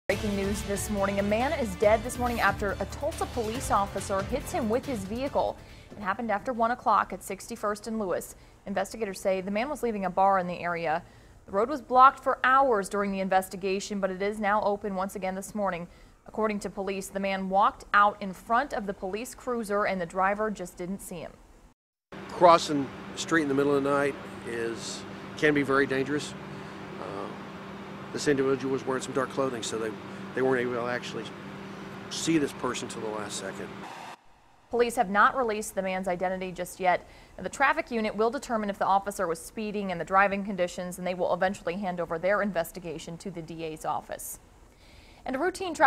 TPD officer hits and kills pedestrian
Police say officer didn't see the man when he hit him with his car.